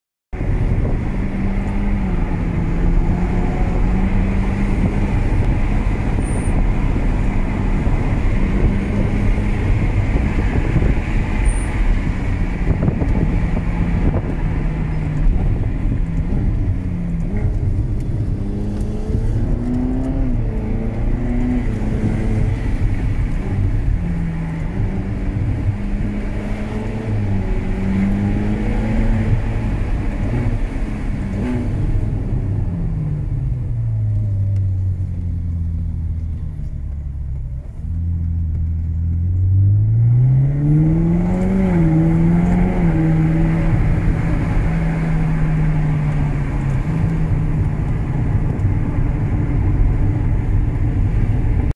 Ferrari California - Bergfahrt
Schon fast artgerechte Haltung.
Baujahr 2012 (das Auto natürlich).
Bergfahrt in der Ostschweiz; kleiner Ausschnitt ...